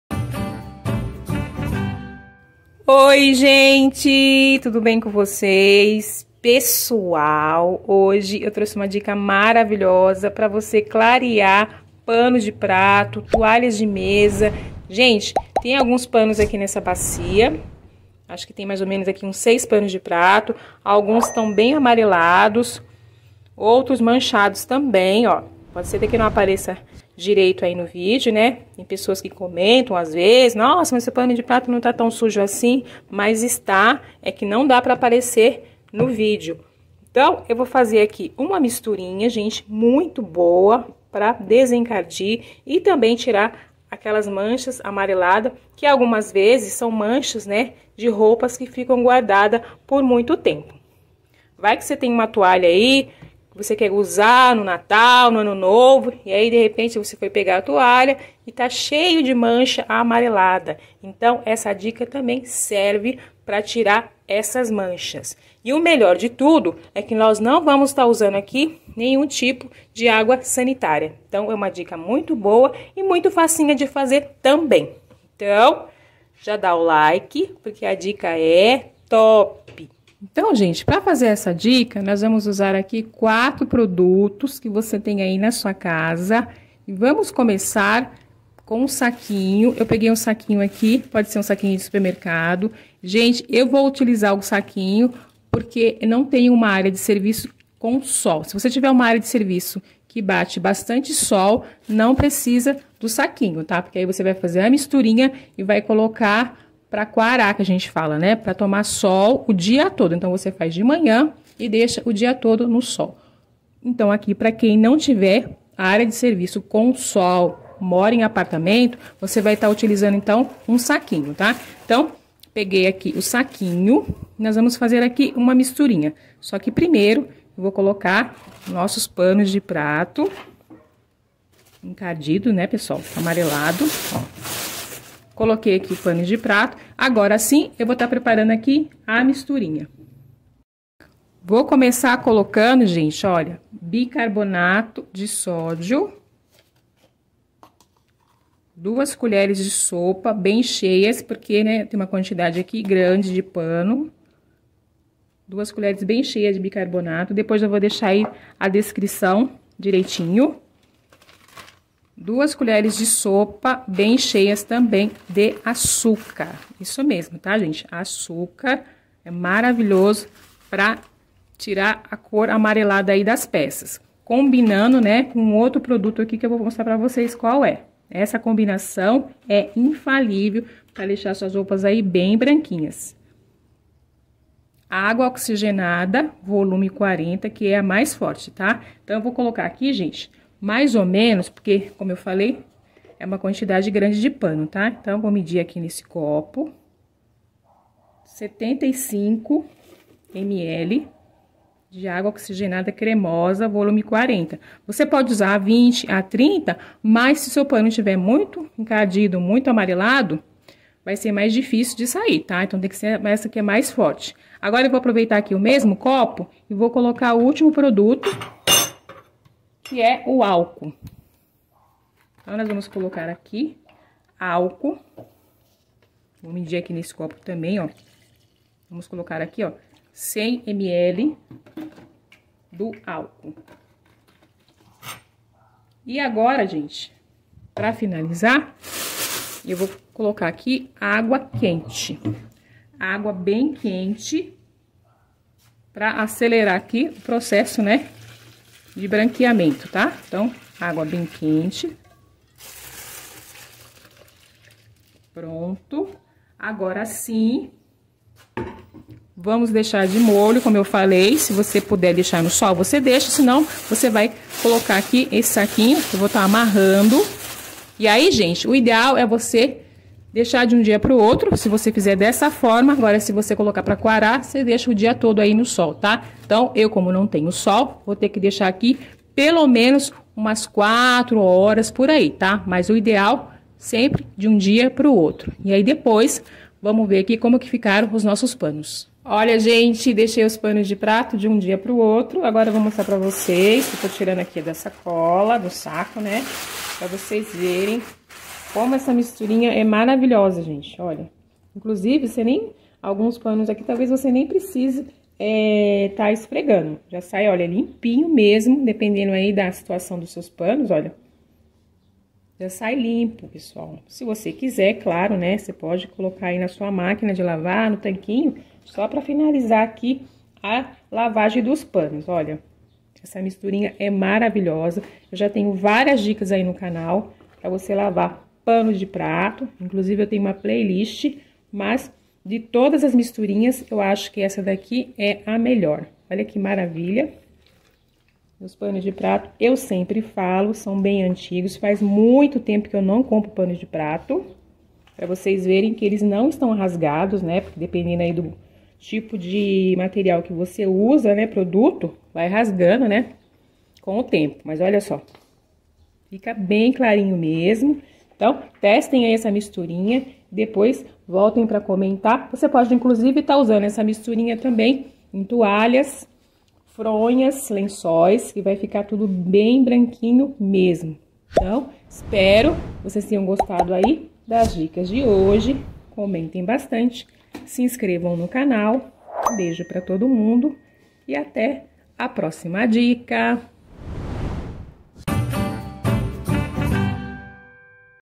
PAREI DE USAR CLORO 😀 SÓ USO AÇÚCAR e ÁGUA OXIGENADA😲 PANOS DE PRATOS FICAM BRANQUINHOS sem AMARELAR
INGREDIENTES DA MISTURINHA PRA DESENCARDIR PANO DE PRATO:
-2 colheres (sopa) de açúcar
-2 colheres (sopa) bicarbonato de sódio
-75 ml de água oxigenada (vol. 40)
-100 ml de álcool
-água bem quente (suficiente pra cobrir todos os panos de prato)